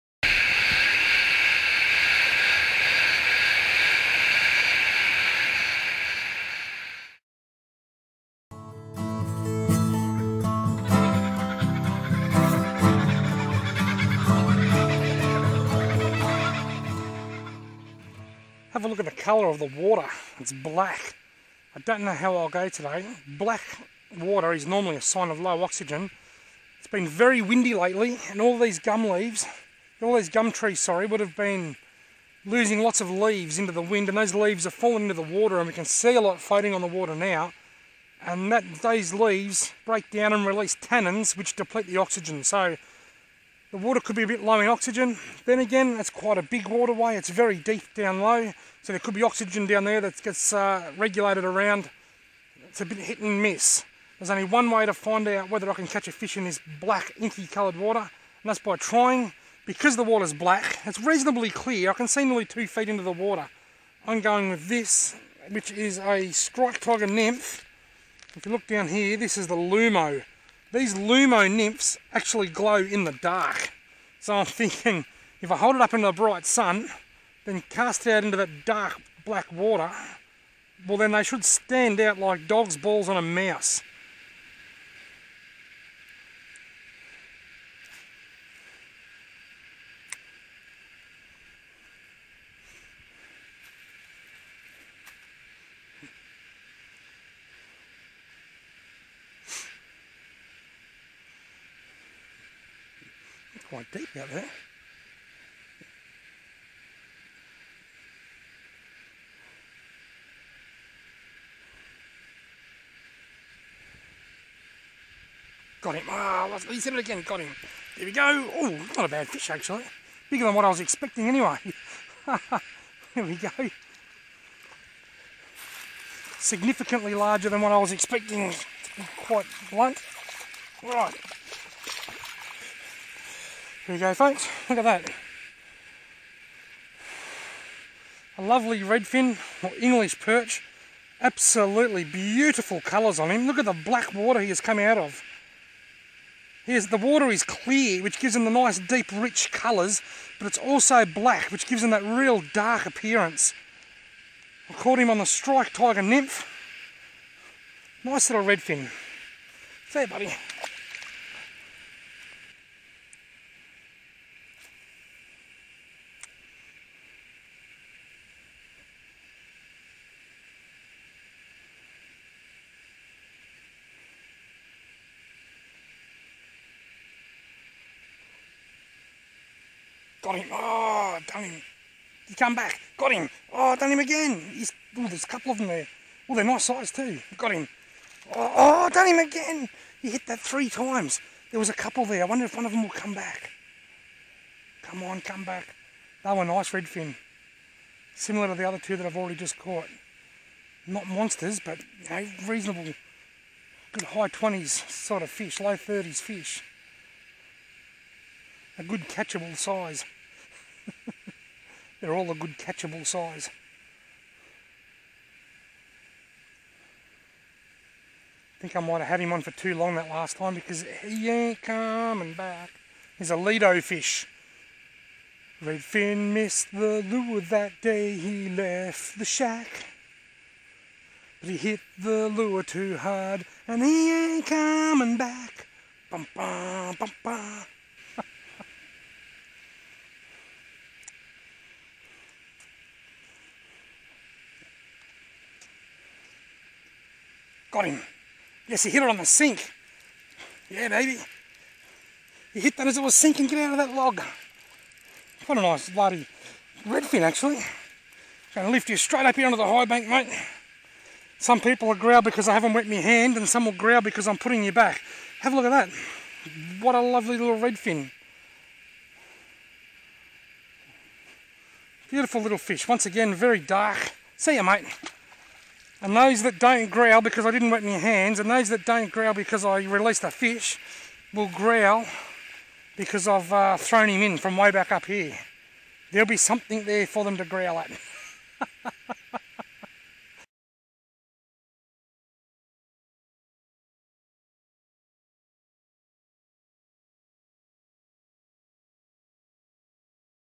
Black water redfin perch
There's black water, and there's blackwater.  Although related they are different. Redfin perch can survive in black water, which is low in oxygen, however they can not survive blackwater events and they are almost totally deprived of oxygen.
Blackwater events occur when stale and stagnent water from billabongs and lagoons floods back into river systems. Blackwater events usually result in fish kills for any species that breaths oxygen from the water. Some species, such as European carp can draw oxygen from the air which makes them much more better equipped to survive these harsh events.
 Redfin perch like oxygen, and get it from the water meaning that they can not survive a toxic black water event.
 In case you are wondering why I call redfin, redfin perch, it is because these fish are only called redfin here in Australia. Sometimes we call then English perch, but usually just redfin. In other parts of the world they are just called perch. So by referring to them as redfin perch, it not only makes the title and description clearer for people viewing from overseas to understand but it also helps my videos to rank better in search engines.